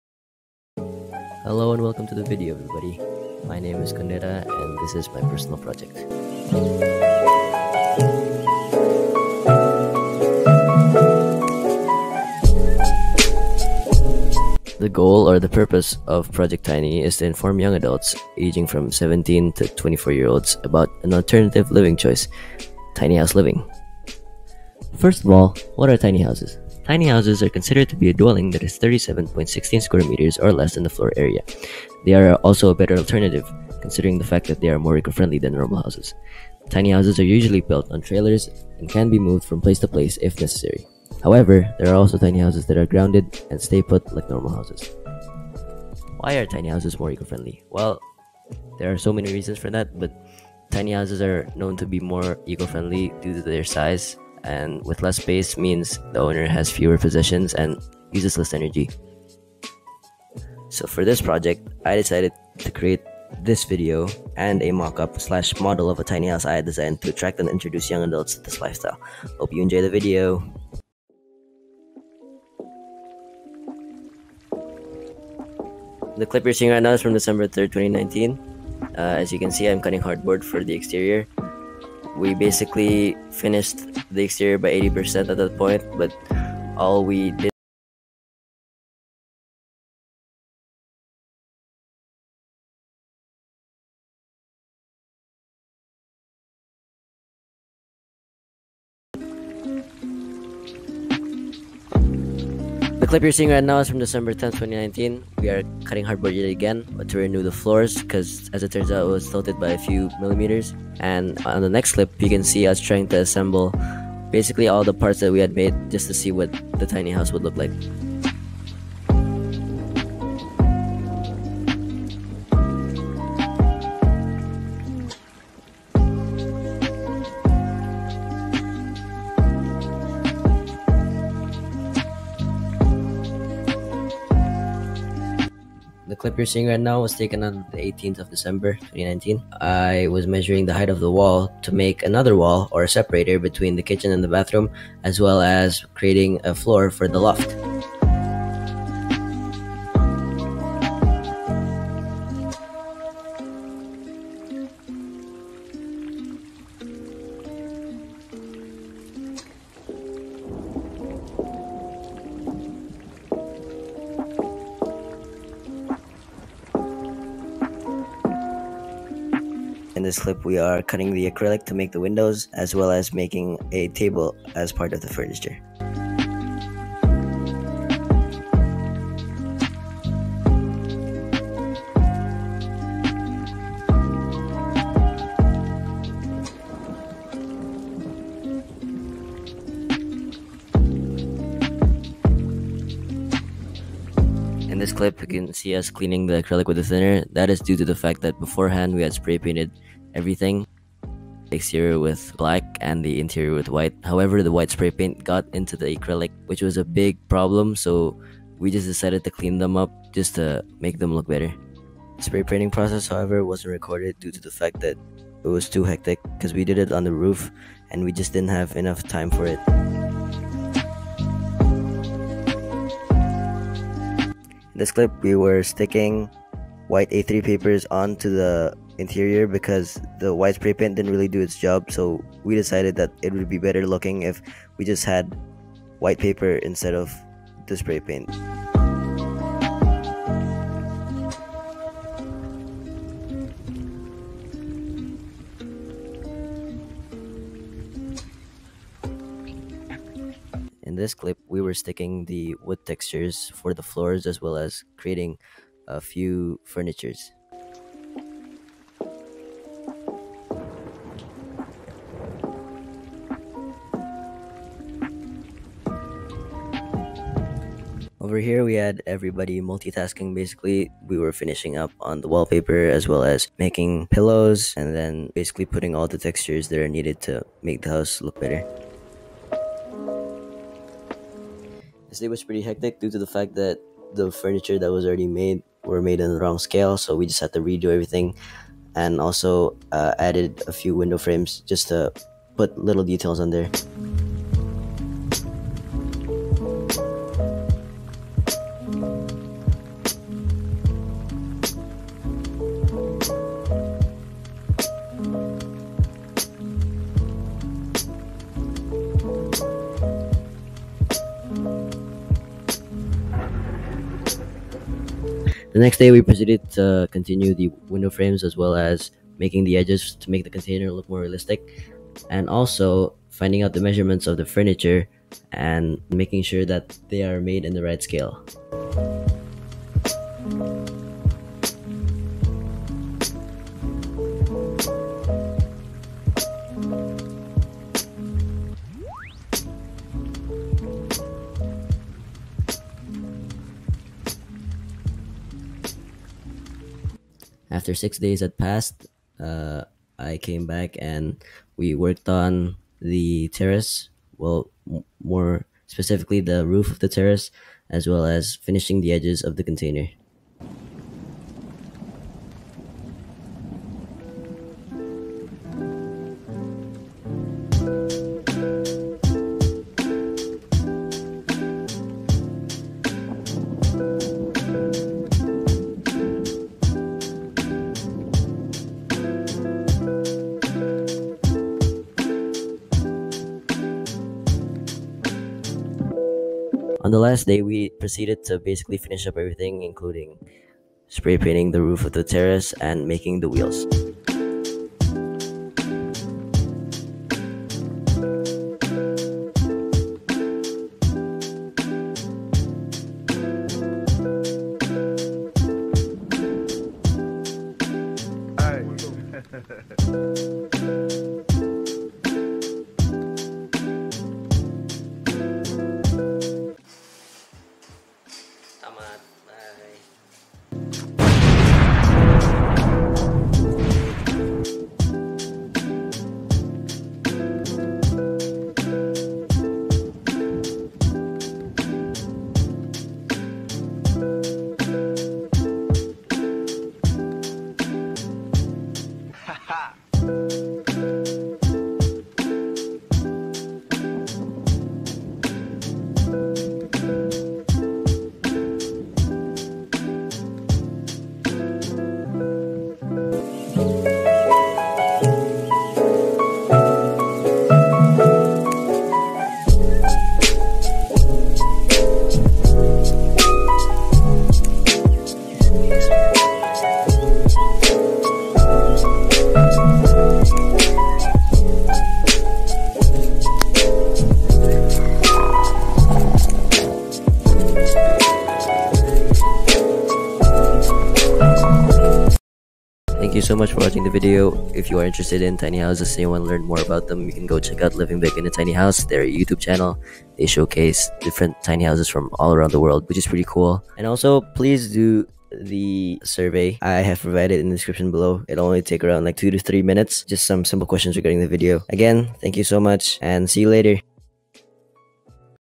Kundera Aqsa Luqman - Project Tiny
After his discovery about tiny houses in YouTube last summer holidays, Kundera found the urge to share the knowledge about this alternative living lifestyle. Through “Project Tiny”, Kundera expands his research and practical skills throughout the process to create a tiny house model, as his final project.

He aims to expose tiny house as one of the alternatives to live in an eco-friendly way as he observes the impact of decision-making on humankind and the environment. He targeted people in age 17-24 years old as his target audience, to impose a conscious future option for younger generation. To create an accurate, presentable, and fully functional tiny house model, Kundera did several in-depth interviews with professional architects, environment analyst, and assisted by an architect while creating his tiny house model.